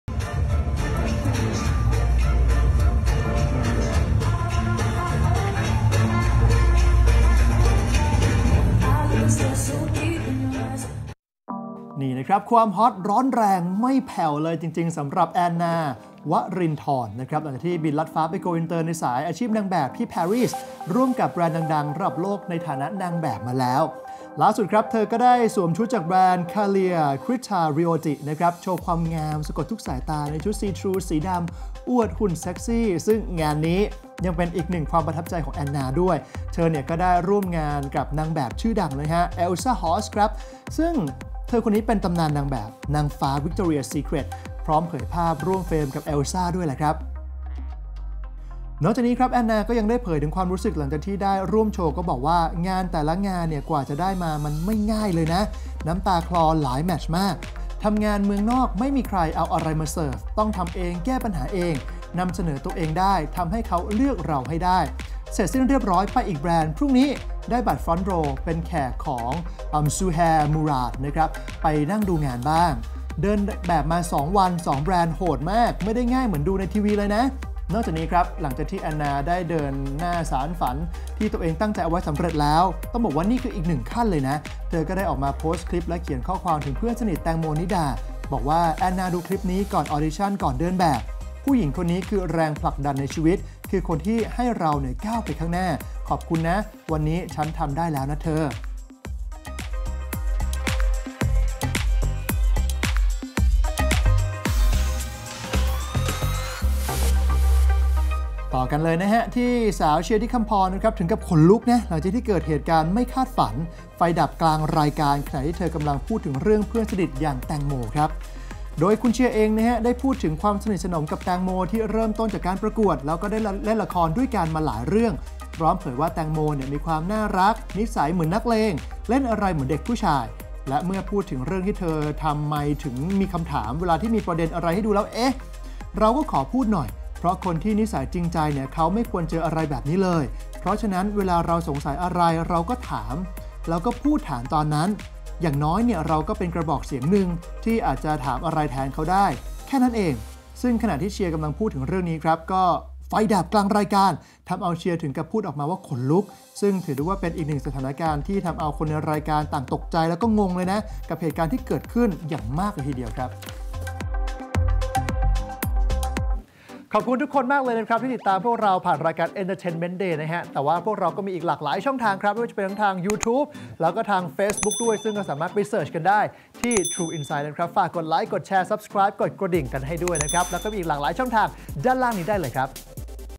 เฉิดฉายต่อเนื่อง "แอนนา" ร่วมโชว์แบรนด์ดัง กระทบไหล่ "Elsa Hosk"
Entertainment Day_060765

ความฮอตร้อนแรงไม่มีแผ่วเลยจริงๆ สำหรับ แอนนา วรินทร  หลังบินลัดฟ้าไปโกอินเตอร์ในสายอาชีพนางแบบ ณ กรุงปารีส ร่วมงานกับแบรนด์ดังระดับโลกในฐานะนางแบบ ล่าสุดเธอมาในชุดจากแบรนด์ Celia Kritharioti โชว์ความงามสะกดทุกสายตาในชุดซีทรูสีดำอวดหุ่นเซ็กซี่ ซึ่งงานนี้ยังเป็นอีกหนึ่งความประทับใจของแอนนา เมื่อเธอได้ร่วมเดินกับนางแบบชื่อดังอย่าง Elsa Hosk ตำนานนางฟ้า Victoria's Secret พร้อมเผยภาพร่วมเฟรมกับ  Elsa Hosk อีกด้วย